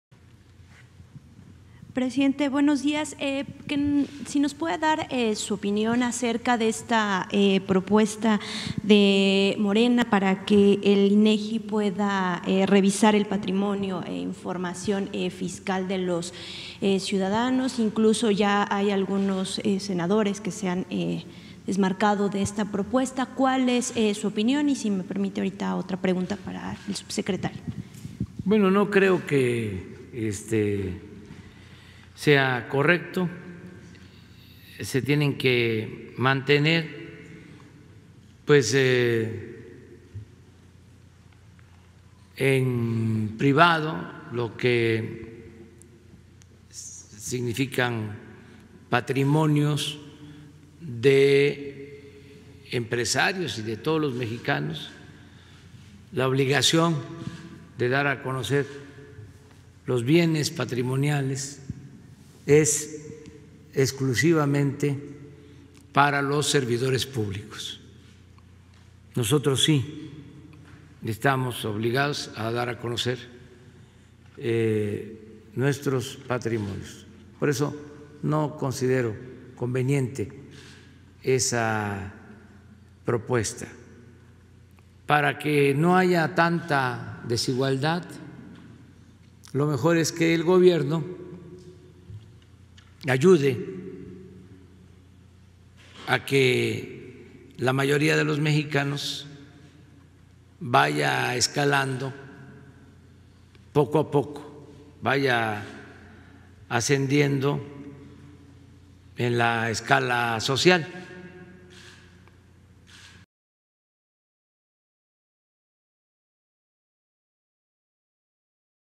AMLO rechaza propuesta del presidente de Morena Alfonso Ramirez
El presidente de México López Obrador (AMLO) consideró inconveniente la propuesta del presidente de Morena, Alfonso Ramírez Cuéllar, para que el Instituto Nacional de Estadística y Geografía (INEGI) mida la riqueza en México, pues afirmó que se tiene que mantener en privado el patrimonio de todos los ciudadanos y sólo los servidores públicos están obligados a darlo a conocer.